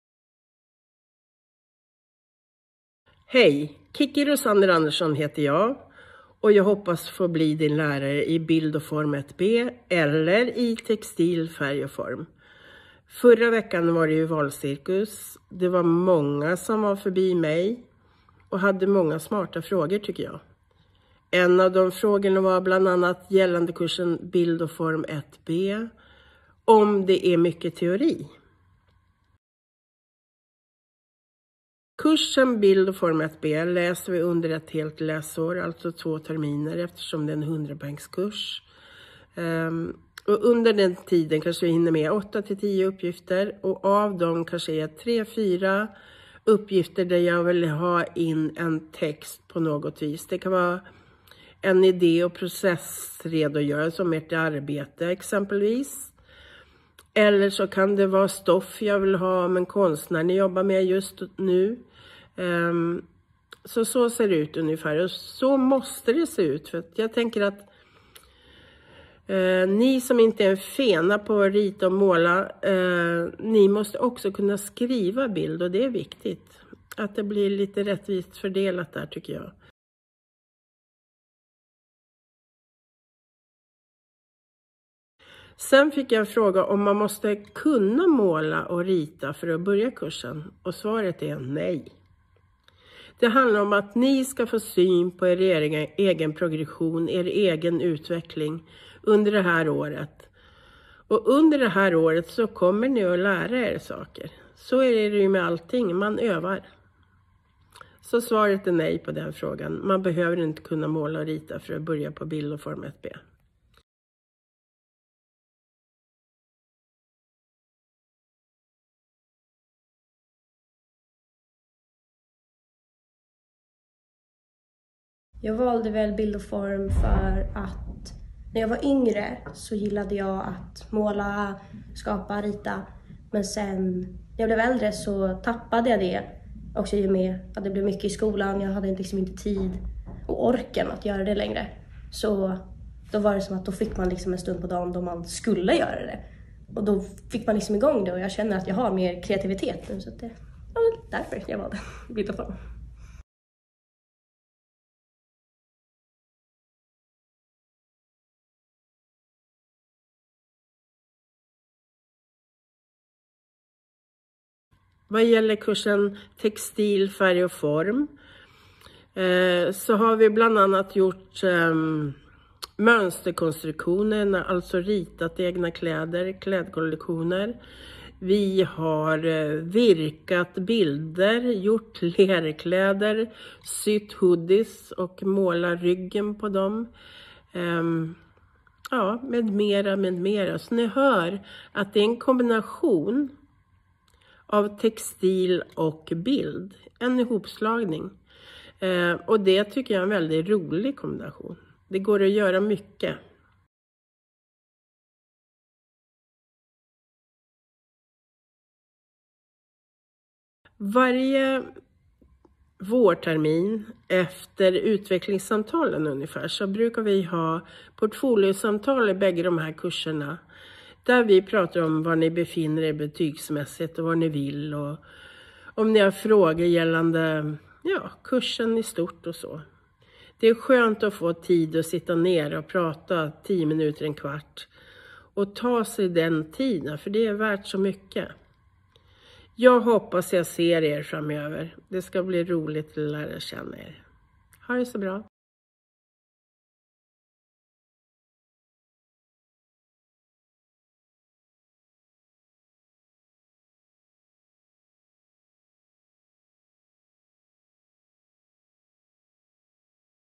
Bild och form 1b & Textil färg och form
Grundläggande kompositionstraditioner för två- och tredimensionella bilder samt färglära. Färdigheter i att framställa bilder med hjälp av olika material, tekniker och konstnärliga uttryckssätt, såväl med traditionella material som i nya konstformer och medier.